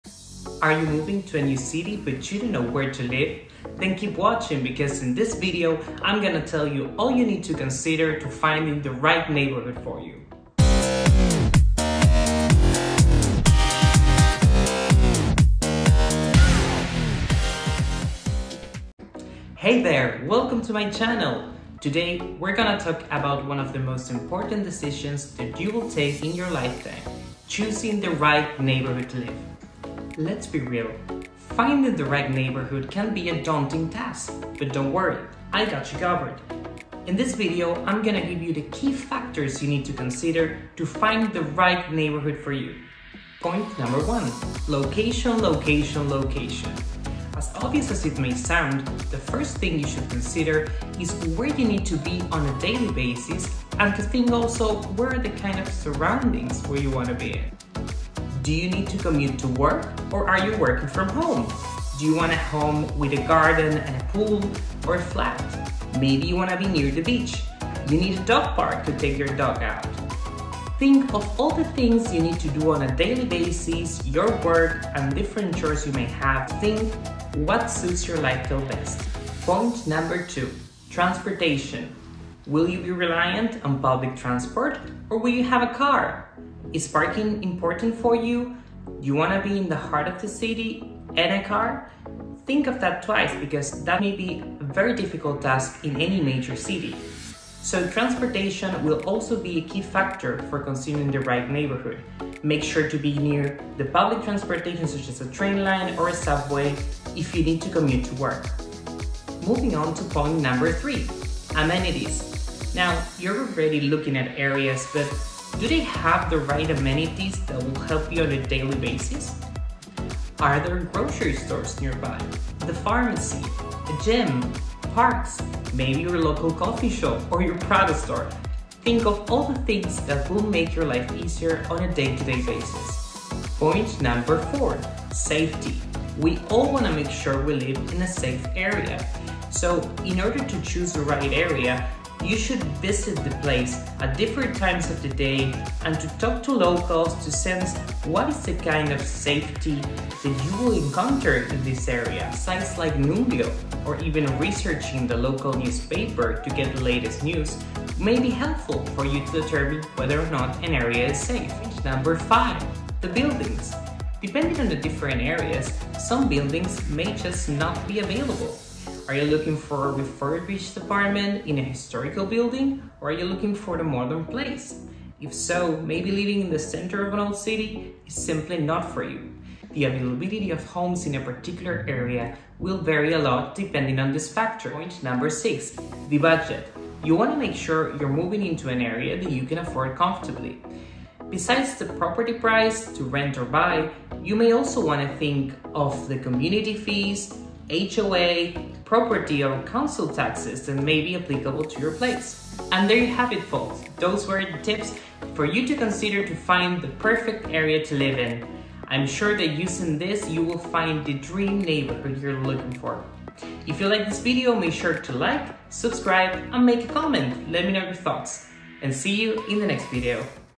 How to Find the Right Neighbourhood to Live | TOP TIPS YOU NEED TO KNOW
In this video you will get the TOP TIPS on finding the right neighbourhood to live abroad as an expat or if you are moving to a new city you just don't know well enough.

0:00 Intro
0:45 1.Location
1:20 2.Transportation
1:50 3.Amenities
2:14 4.Safety
2:42 5.Buildings